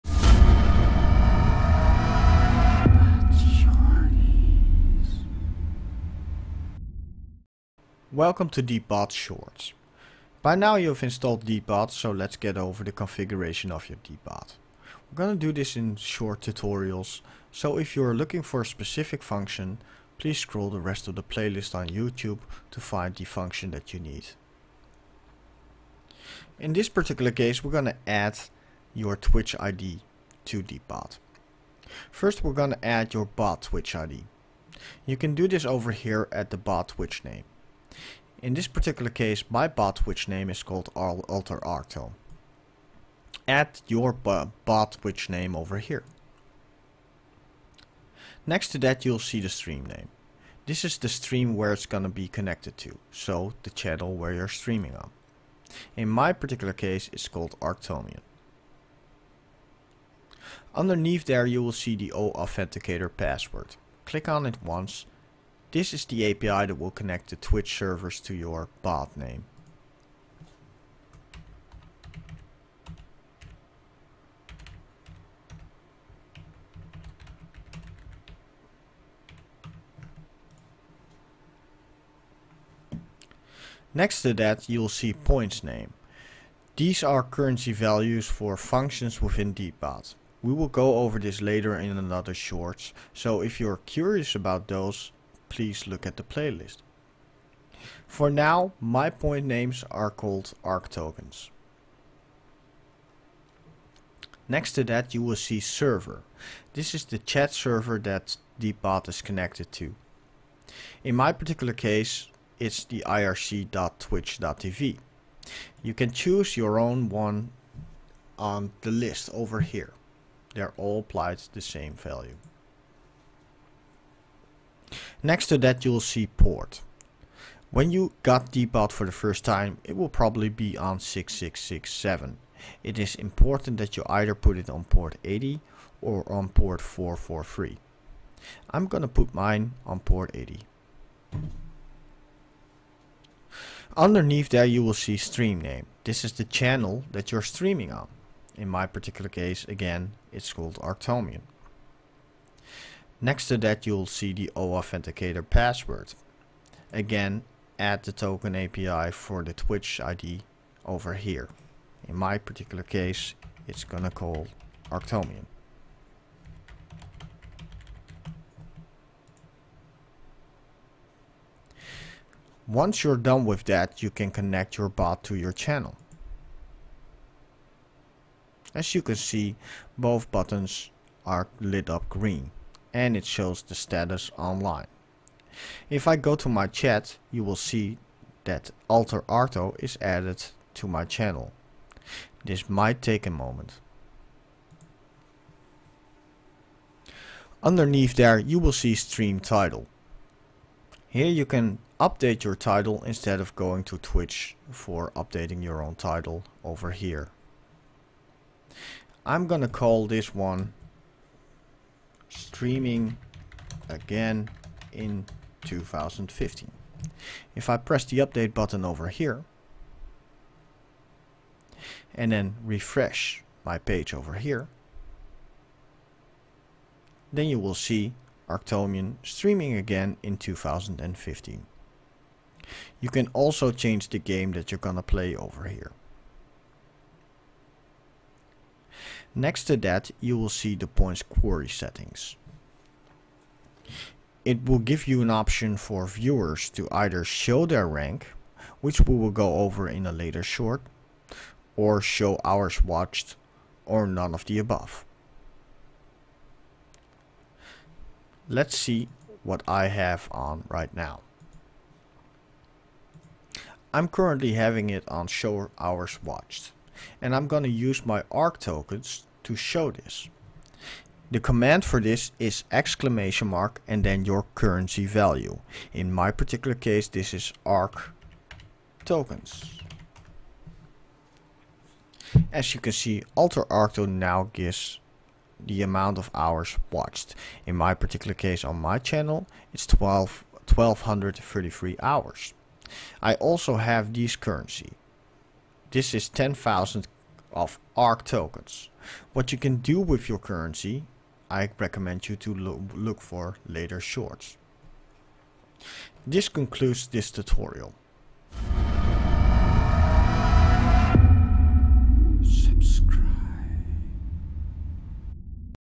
#1 Deepbot Short'Z: Stream Configuration
Short'z are 5 min tutorials going over 1 or 2 specific functions in Deepbot streamer assistant.

~ Bot login
~ Stream login
~ Chat server
~ Port
~ Stream title
~ Points Query Settings